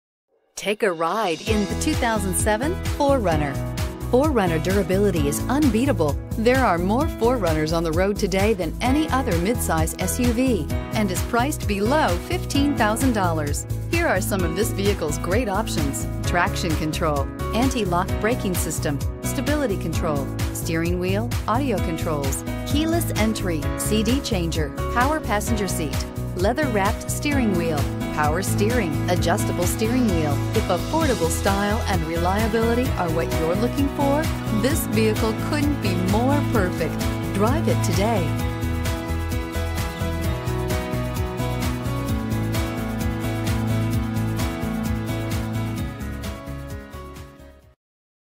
2007 Toyota 4Runner 18HPI23A
2007 TOYOTA 4RUNNER Tyler, TX
Stock# 18HPI23A

Jack O'Diamonds
2500 Wsw Loop 323

PRICED TO SELL QUICKLY! Research suggests it will not be available for long. -Oil Changed -New Rear Brakes -State Inspection Completed -New Wiper Blades -New Cabin Air Filter -Fully Detailed This model has many valuable options -Leather seats -Aux. Audio Input -Heated Front Seats -Heated Mirrors -Auto Climate Control -Multi-Disc Changer -Automatic Headlights -Fog Lights -Rear Wheel Drive -Multi-Zone Air Conditioning -Security System -Garage Door Opener -Power Locks -Keyless Entry -Power Windows -Seat Memory -Steering Wheel Controls -Cruise Control -Running Boards -Leather Steering Wheel -Roof Rack Automatic Transmission -Rear Bench Seats -Tire Pressure Monitors On top of that, it has many safety features NHTSA 5 Star Crash Rating! -Brake Assist -Traction Control -Stability Control Call to confirm availability and schedule a no-obligation test drive! We are located at 2500 W SW Loop 323, Tyler, TX 75701.